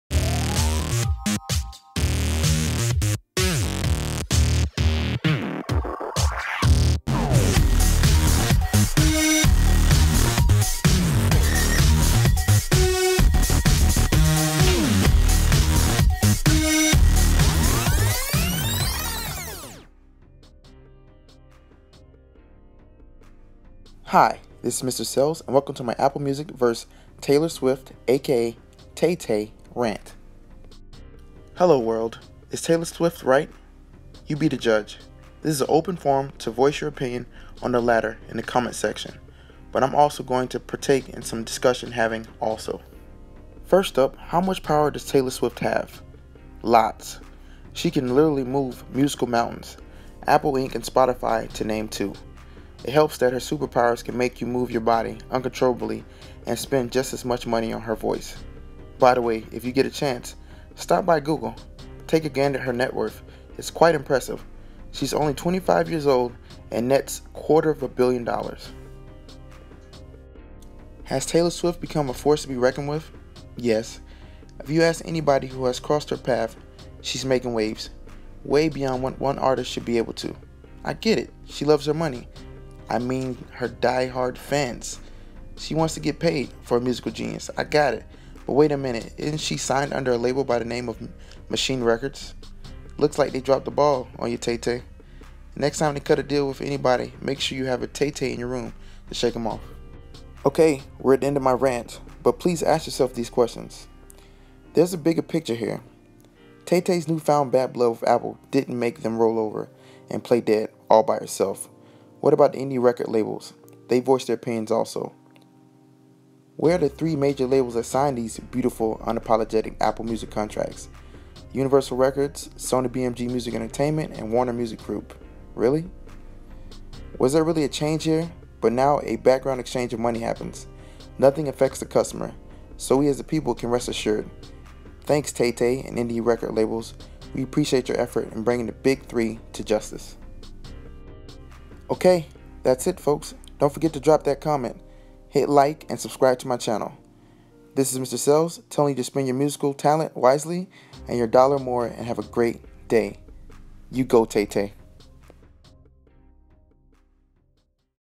Apple Music VS Taylor Swift AKA TAY TAY Rant
Apple Music VS Taylor Swift AKA Tay Tay Rant video is me painting the big picture of Apple didn't wake up one day and just say hey we are using this music for free for 3 months, the major labels had to sign off on this. Where were these artist when all this happened? A link to Tay Tays letter to Apple